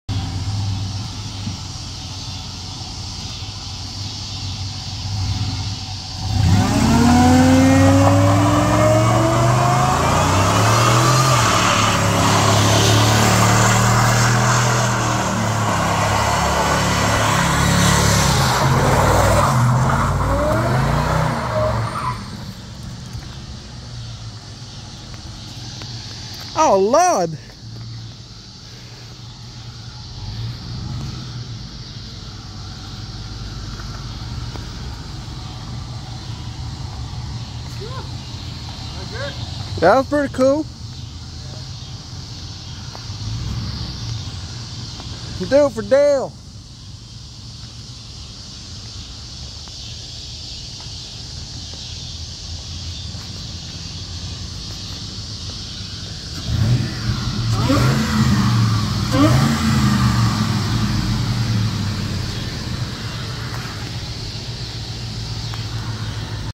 f150 supercharged
2003 f150 hd 100% stock

the toyo proxes are quite the smoke show!!